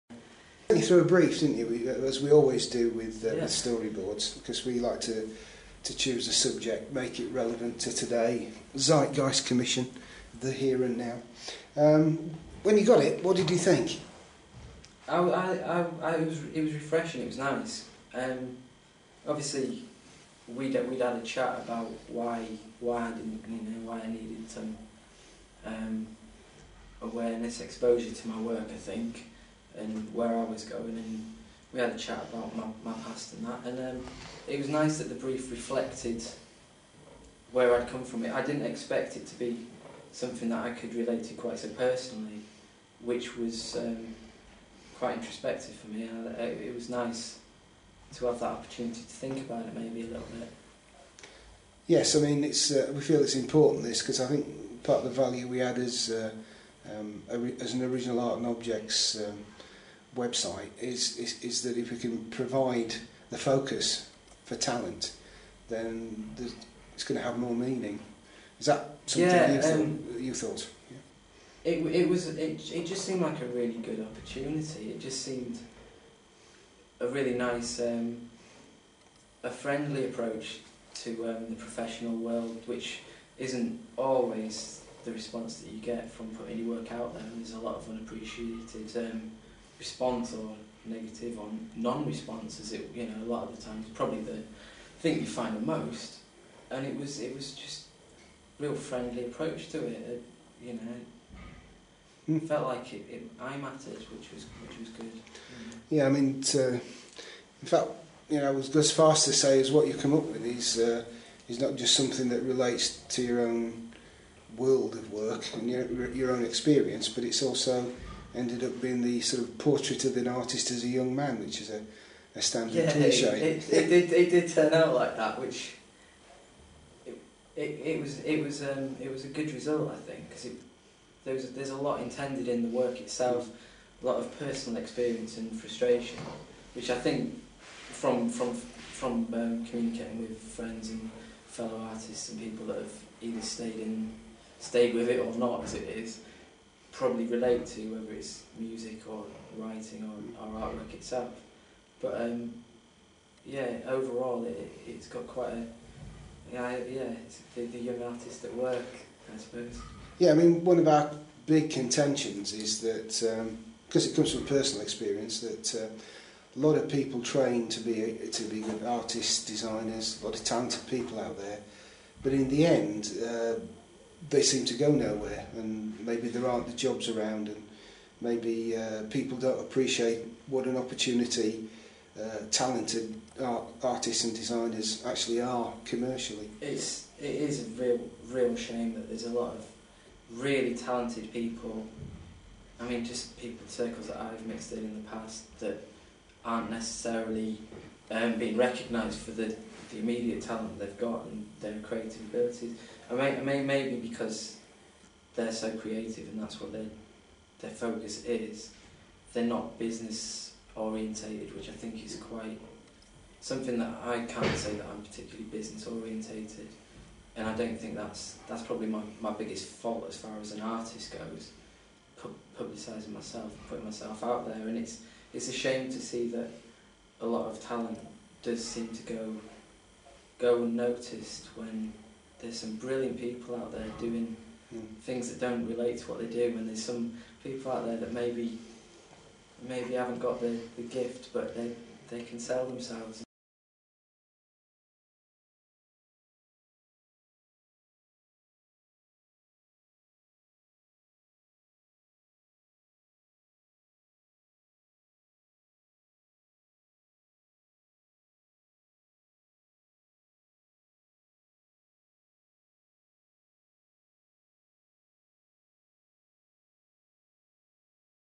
The World of Work
artist Jonathan Brier discusses his latest painting - The World of Work. The piece encapsulates his experience since leaving art college and the difficulties arts gradutes have in making a living in their chosen field. The painting itself can be seen at the end of the video